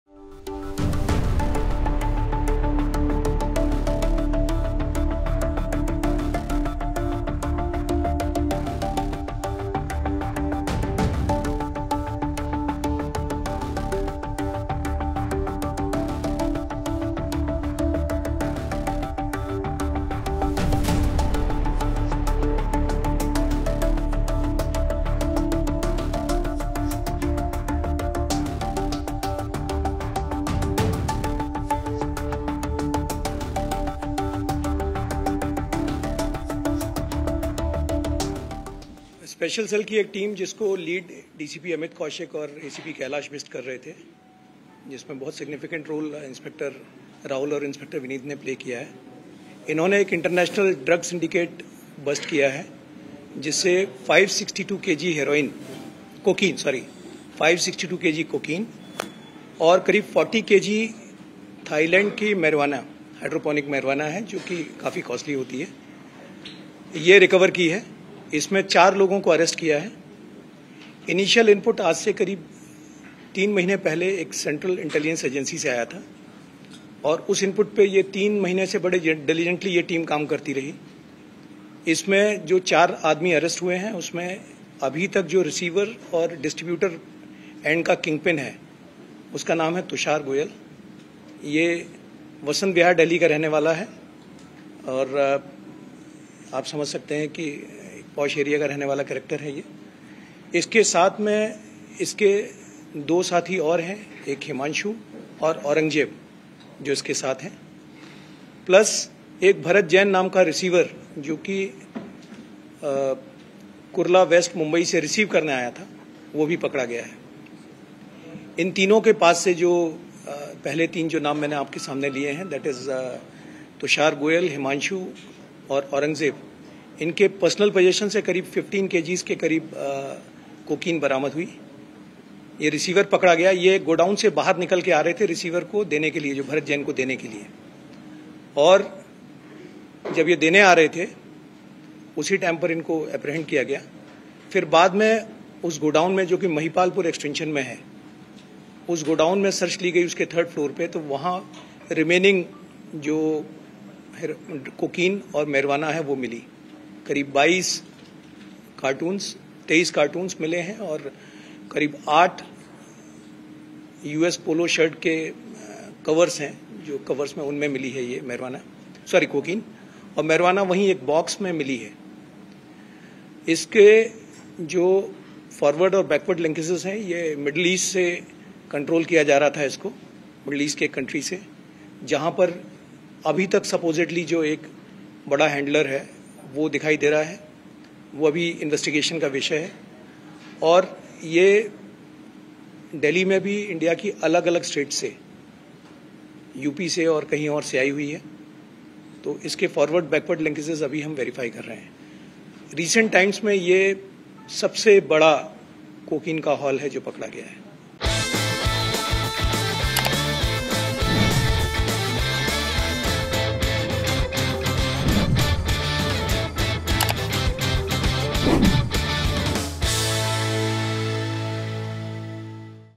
Delhi Police Busts Biggest Drug Cartel, Seizes 560 Kg Cocaine Worth Rs 2,000 Crore
In a massive crackdown on international drug syndicates, the Delhi Police Special Cell has carried out the largest drug bust in the capital’s history, seizing over 560 kg of cocaine valued at more than Rs 2,000 crore. The raid, led by DCP Amit Kaushik and ACP Kailash Bisht, also resulted in the arrest of four individuals linked to the syndicate—Tushar Goyal, Himanshu, Aurangzeb, and Bharat Jain. Along with the cocaine, the police recovered 40 lakh worth of Thai marijuana. This major consignment of narcotics was allegedly intended for high-profile parties. Authorities are investigating further to uncover more details about the cartel and its operations. Additional CP of the Delhi Police Special Cell, Pramod Singh Kushwaha, stated, "A Special Cell team, led by DCP Amit Kaushik and ACP Kailash Bisht, successfully dismantled an international drug syndicate, resulting in the arrest of four individuals."